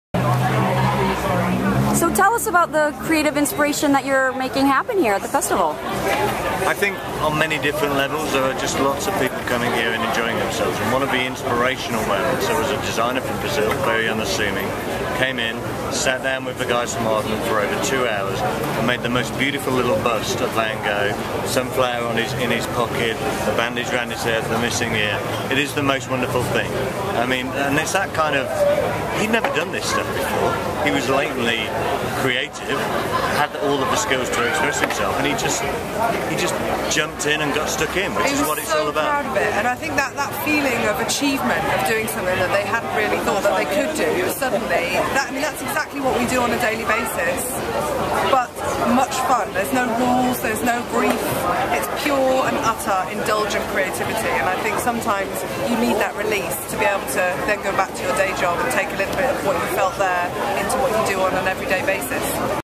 The Brand Union and Lambie-Nairn on Play, Make, Master
Sue Daun of The Brand Union and Adrian Burton of Lambie-Nairn discuss the importance of playfulness in the creative process, as part of their Play, Make, Master exhibit at the Cannes Lions International Festival of Creativity.